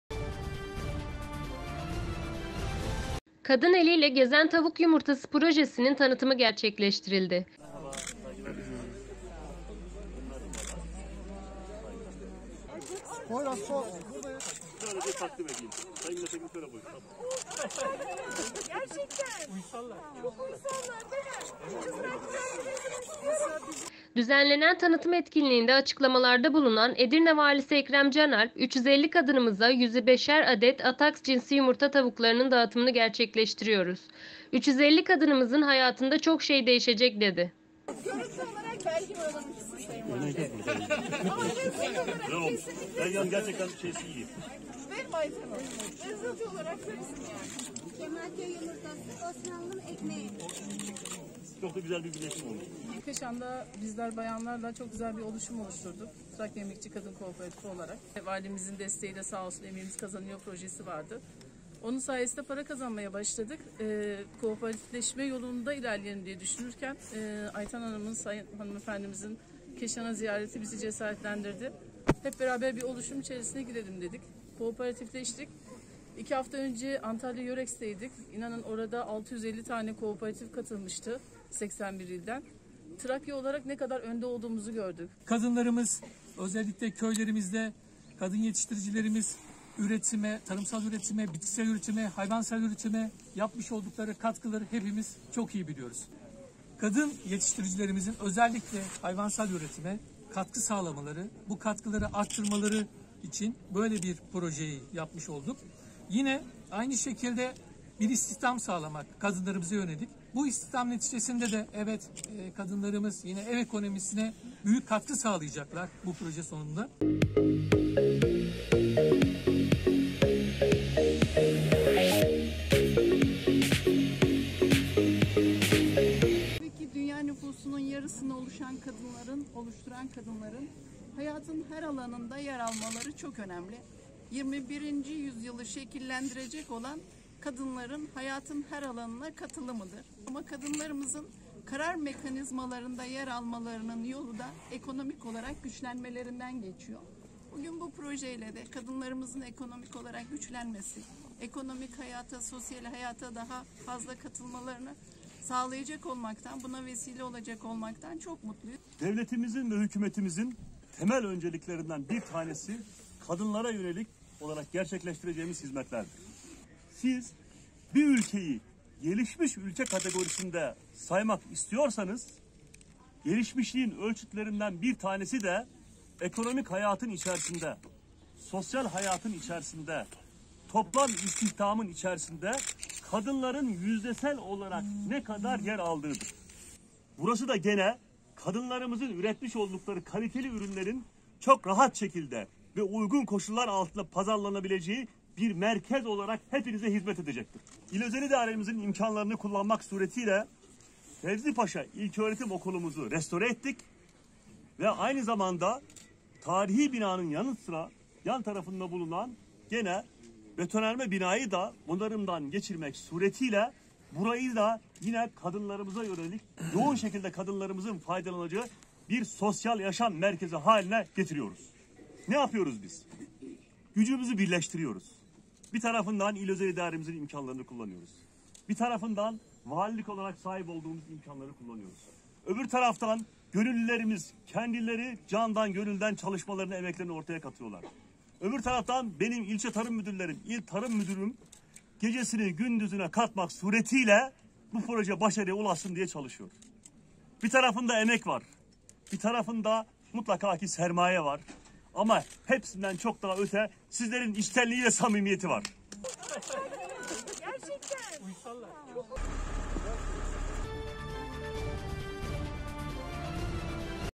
Proje Kapsamında 36 Bin 750 Tavuk Dağıtılacak
‘Kadın Eliyle Gezen Tavuk Yumurtası’ projesinin tanıtımı gerçekleştirildi. Düzenlenen tanıtım etkinliğinde açıklamalarda bulunan Edirne Valisi Ekrem Canalp; “350 kadınımıza 105’er adet Ataks cinsi yumurta tavuklarının dağıtımını gerçekleştiriyoruz. 350 kadınımızın hayatında çok şey değişecek.” dedi.